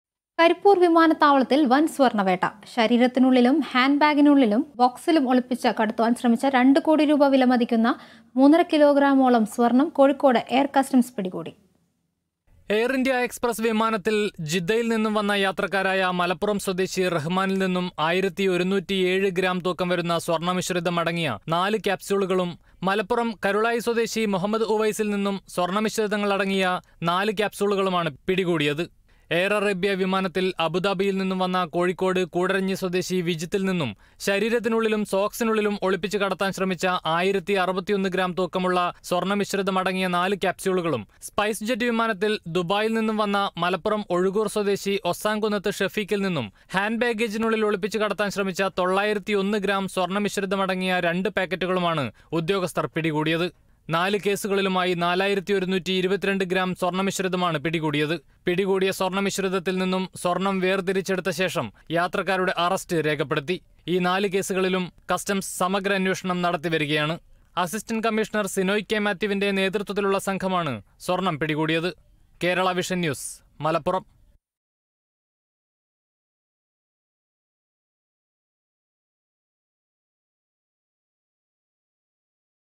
കരിപ്പൂര്‍ വിമാനത്താവളത്തില്‍ വന്‍ സ്വര്‍ണ്ണ വേട്ട; മൂന്നര കിലോഗ്രാമോളം സ്വര്‍ണം പിടികൂടി
കരിപ്പൂര്‍ വിമാനത്താവളത്തില്‍ വന്‍ സ്വര്‍ണ്ണ വേട്ട. ശരീരത്തിനുള്ളിലും ഹാന്‍ഡ് ബാഗിനുള്ളിലും സോക്സിനുള്ളിലും ഒളിപ്പിച്ചു കടത്തുവാന്‍ ശ്രമിച്ച  2 കോടി രൂപ വില മതിക്കുന്ന  മൂന്നര കിലോഗ്രാമോളം സ്വര്‍ണം കോഴിക്കോട് എയര്‍ കസ്റ്റംസ് പിടികൂടി.

About COA